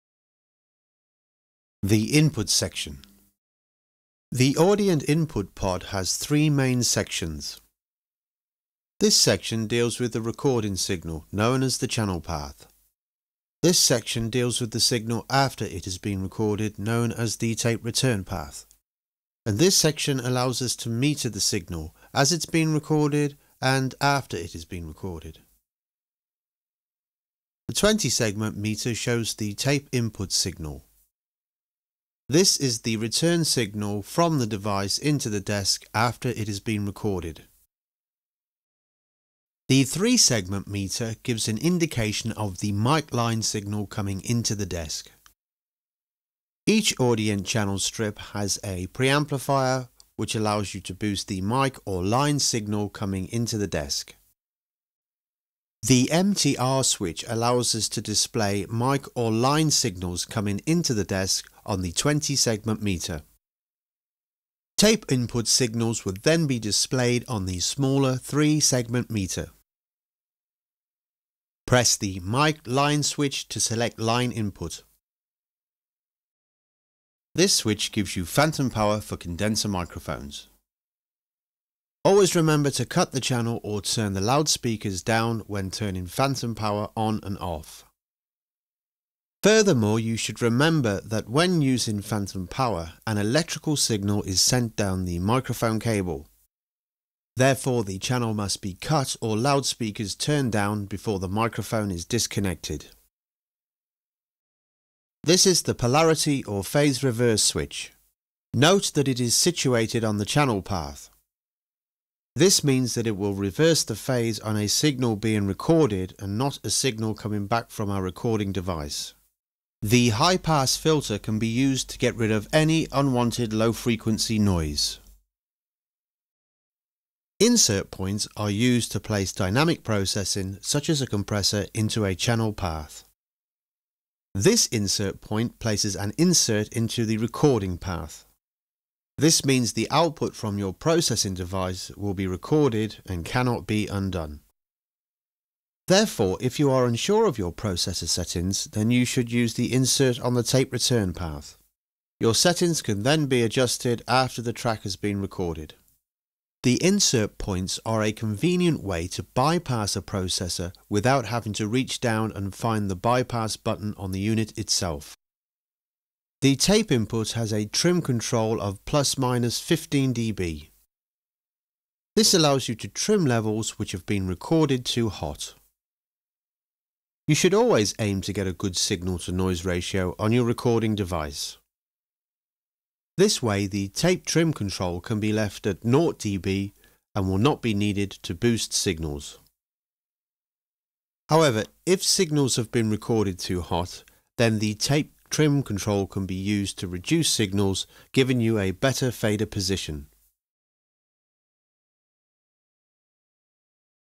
Audient ASP8024 Manual Podcast - The Input Section
The input section is the gateway to the remainder of the signal processing of the console. Note the different colouring used to identify the different signal paths of the pod.